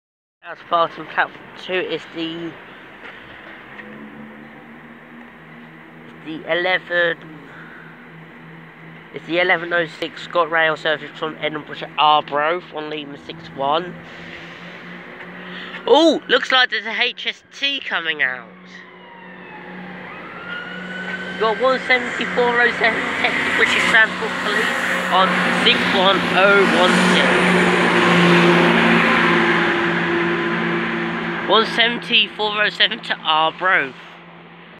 170407 *Text BTP on 61016* departs of P2 at Haymarket 26/10/2019
1L61 11:07
ScotRail Service from
Edinburgh Waverley to Arbroath